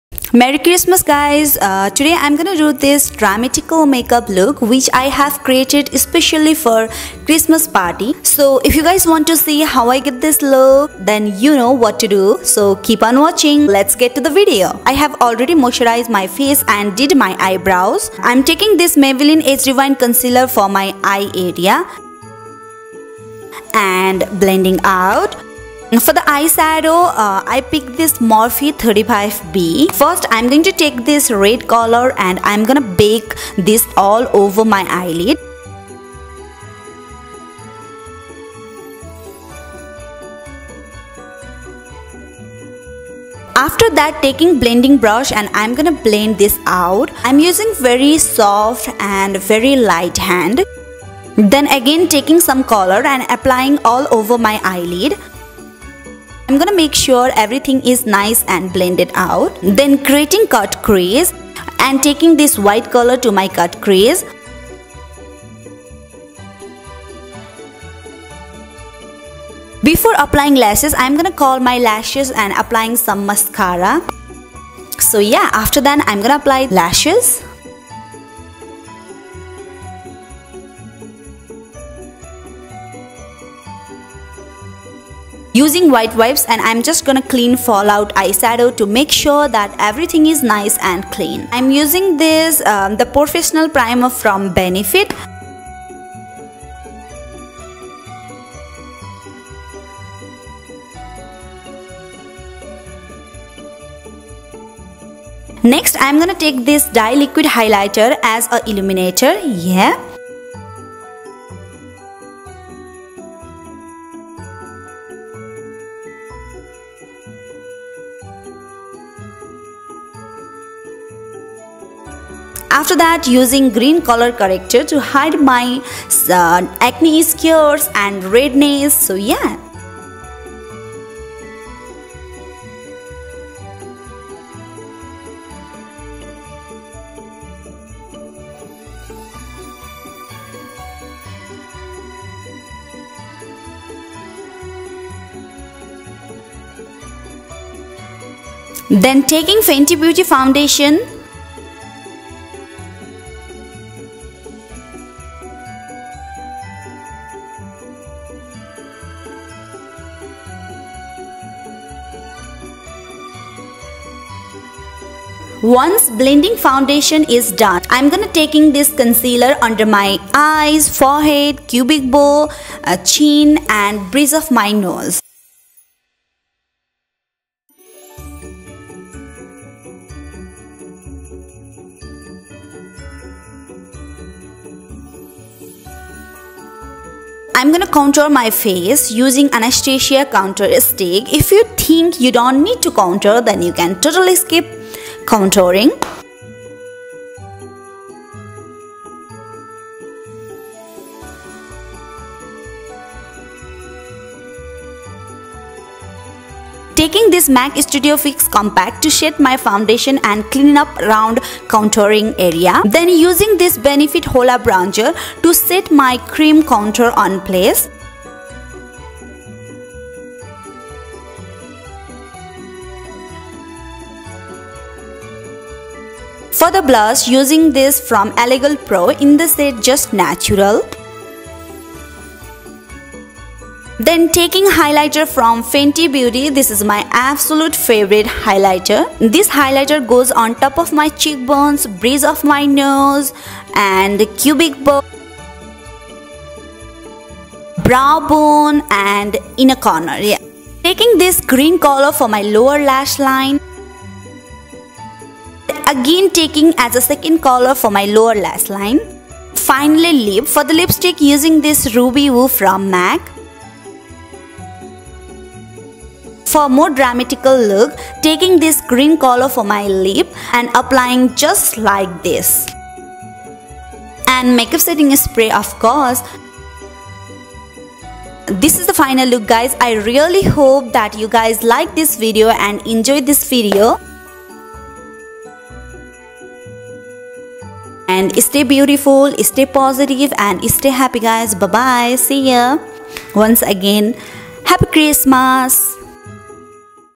Dramatic Christmas Makeup Transformation 2018
Here is the dramatic makeup look for Christmas party. I hope you guys gonna like this video and find this video. If you do then please don't forget to give me a thumbs-up.